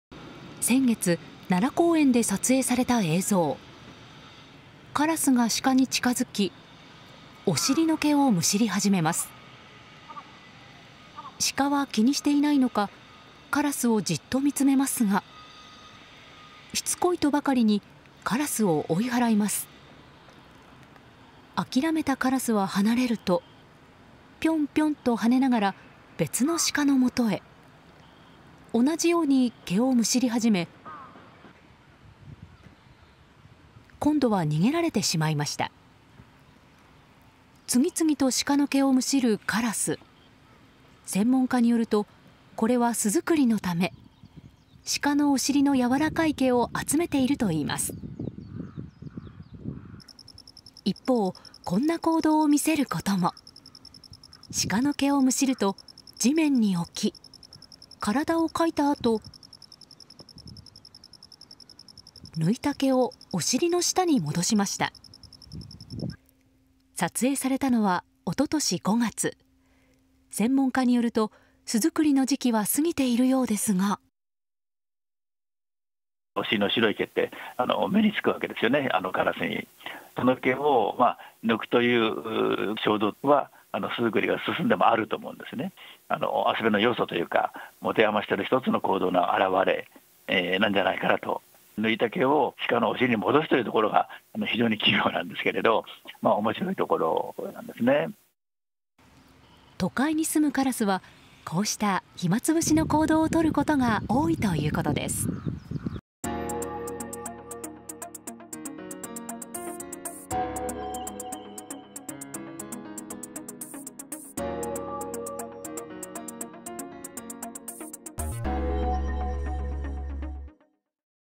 なぜシカの毛むしる？ カラス行動 驚きの理由
3月、奈良公園で撮影された映像。
カラスがシカに近づき、お尻の毛をむしり始めた。

シカは気にしていないのか、カラスをじっと見つめるが、しつこいとばかりにカラスを追いはらう。

諦めたカラスは離れると、ピョンピョンと跳ねながら別のシカの元へ。

同じように毛をむしり始め、今度は逃げられてしまった。

次々とシカの毛をむしるカラス。

専門家によると、これは巣作りのためで、シカのお尻のやわらかい毛を集めているという。

一方、こんな行動を見せることも。

シカの毛をむしると地面に置き、体をかいたあと、抜いた毛をお尻の下に戻した。

撮影されたのは。2021年5月。

専門家によると、巣作りの時期は過ぎているようだが...。

東京大学・樋口広芳名誉教授「お尻の白い毛は、カラスの目につく。毛を抜く衝動は、巣作りが済んでもあると思う。遊びの要素、(ひまを)もてあました行動の現れでは。抜いた毛をシカのお尻に戻すのは、非常に器用で面白い」

都会に住むカラスは、こうした暇つぶしの行動をとることが多いという。